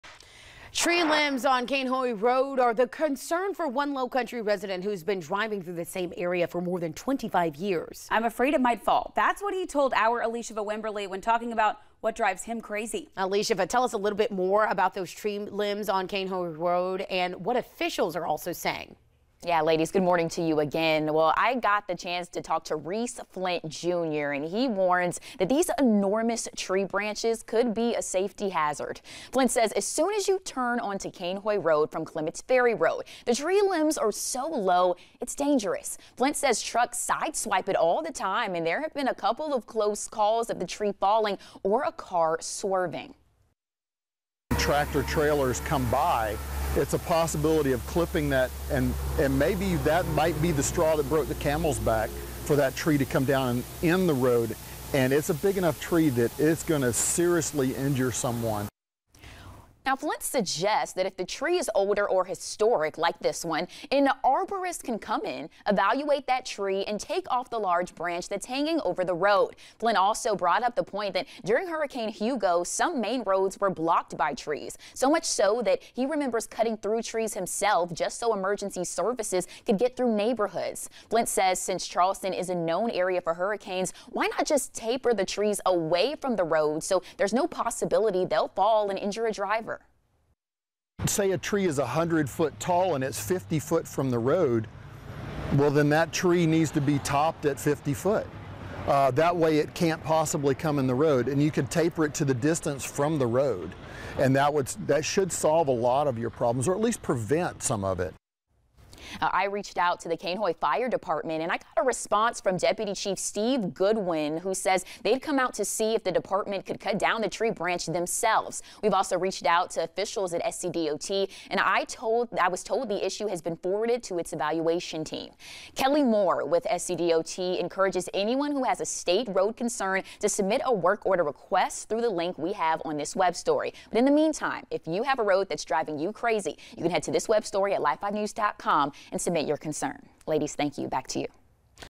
VIDEO: Lowcountry resident concerned tree branches could cause crash
Tree limbs on Cainhoy Road are a concern for one Lowcountry resident who has been driving in the same area for over 25 years.